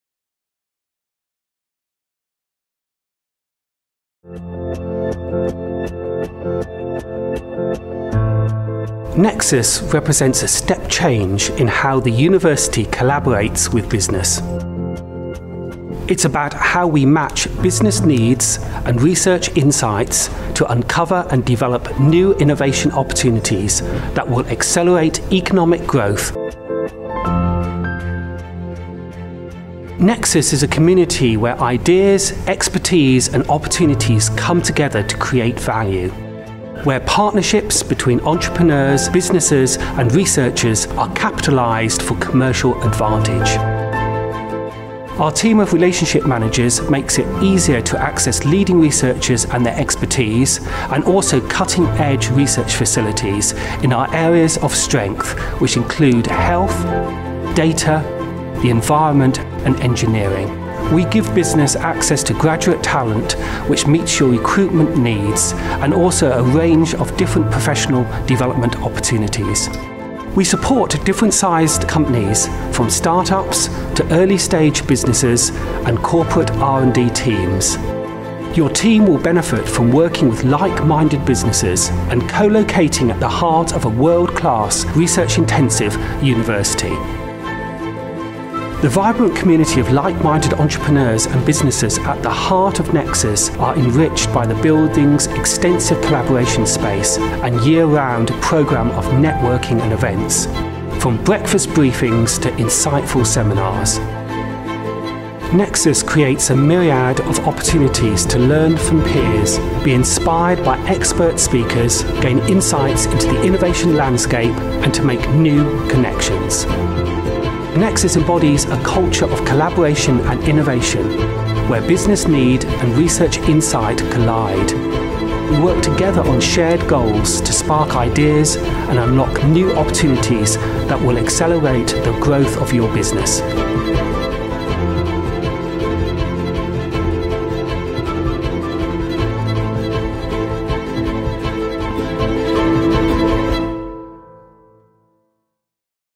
What is Nexus?
Nexus is a vibrant community for innovators and entrepreneurs. We help turn your big ideas into reality.

Our community of high-growth businesses is located across the globe but has its physical base in a state-of-the-art innovation hub on the University of Leeds campus.

In this video Nexus Director, Dr Martin Stow, talks about how we work and the positive impact we have made across Leeds, the region, nationally and internationally.

The Nexus team help our members make the right connections and provide access to research, talent and facilities across the University of Leeds and beyond. Our expert team bridge the gap between business and academia, helping businesses access the knowledge, people and workspace they need to thrive.

Businesses can engage with Nexus in a variety of ways, whether that’s becoming a member of the Nexus community to basing their operations in our high-quality office and lab space.

Learn more about how we have helped our members to innovate and grow